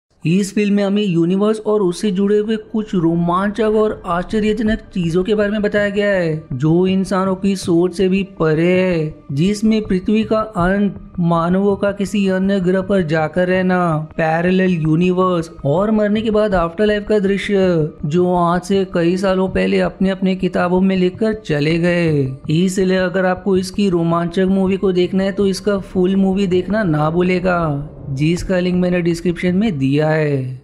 Shorts | An Inventor Vanishes Mysterious After Creating A Device That Will Change The World
Video sachme pasand aajae toh aap JOIN, ya 💲APPLAUD par click karke, Google Pay ya BHIM UPI se pay karke support dikha sakte ho 🙂